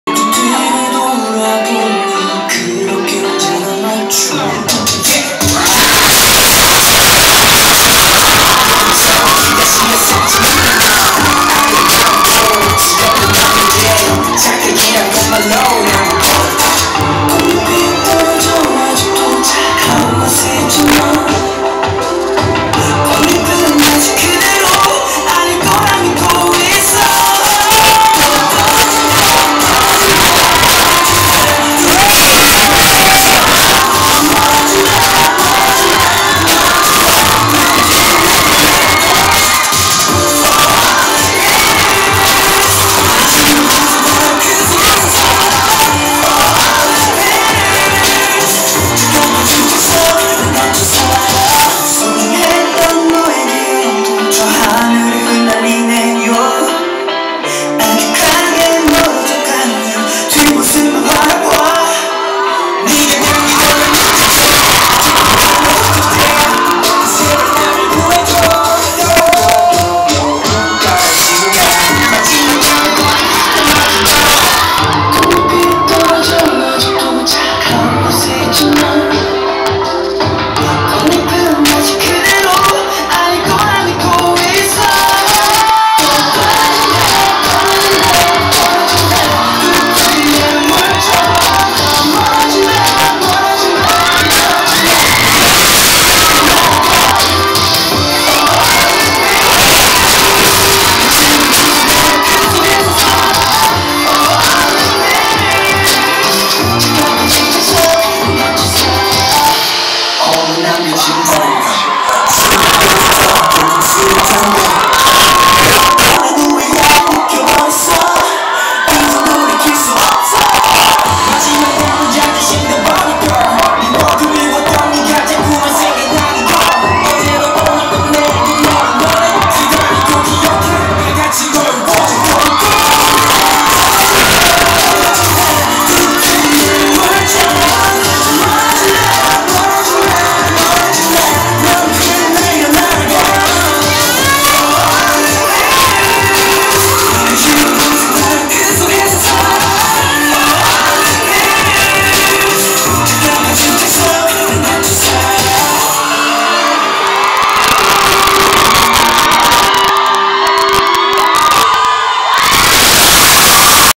Lucente performing for Indian fans
Kpop group Lucente came to Mumbai & pune to perform their fans.
Previous year (in 2017) they promised to come back for their fans & they literally fulfilled their promise by coming back.
Thank you Lucente for coming back to India ❤😍😘 사랑해 Lucente